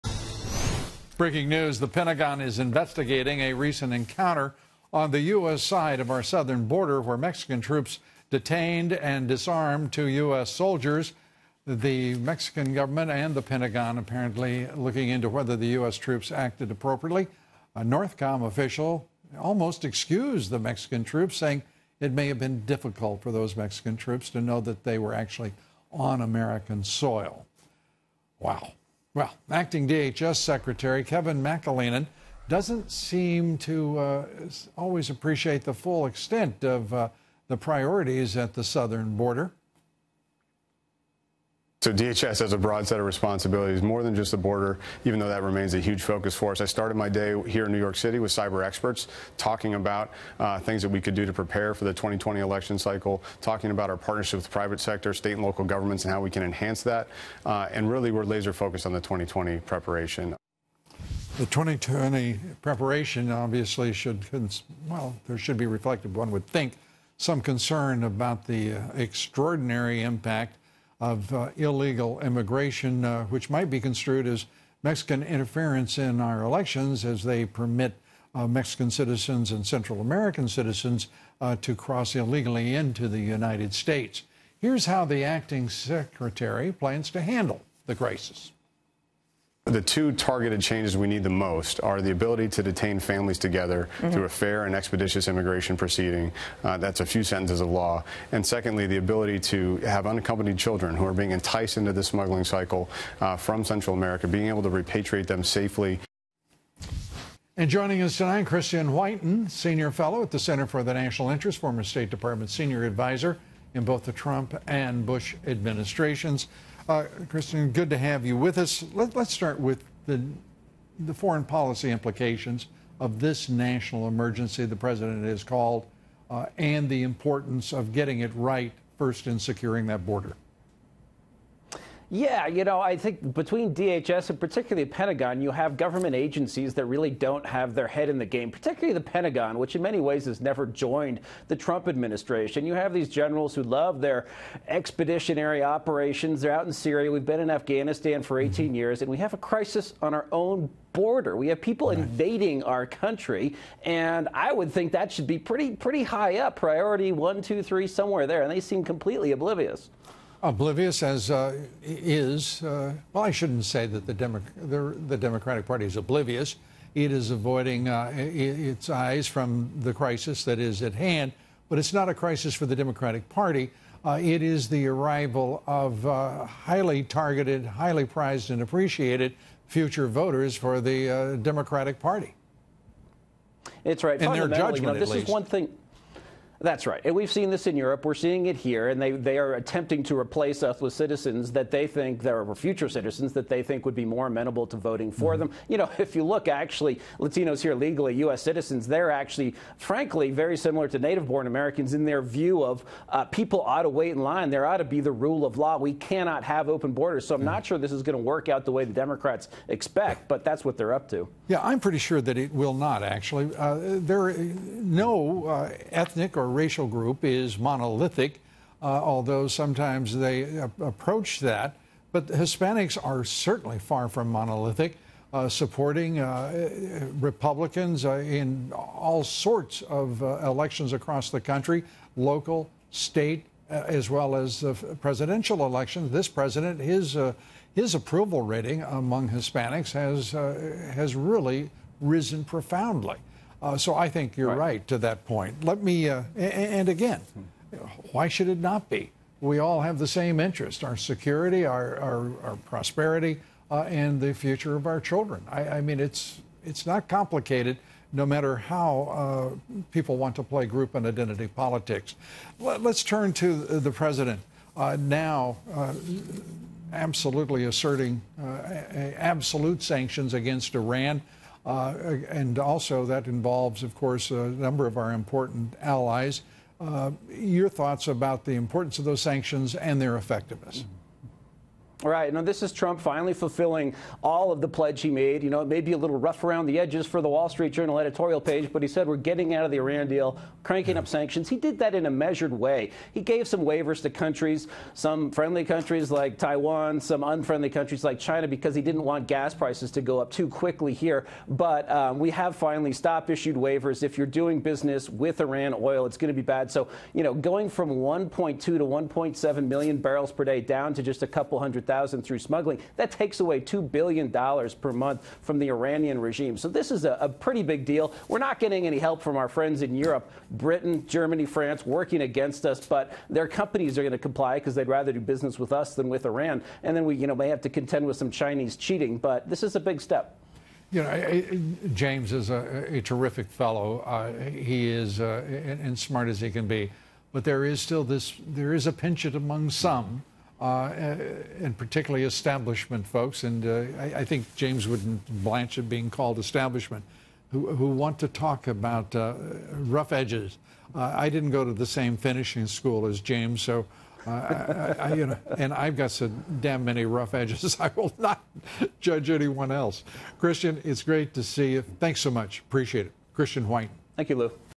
Pentagon investigating American soldiers detained by Mexican troops at U.S. border
Former Trump State Department senior adviser Christian Whiton says that the Pentagon seems “oblivious” to the crisis at the southern border.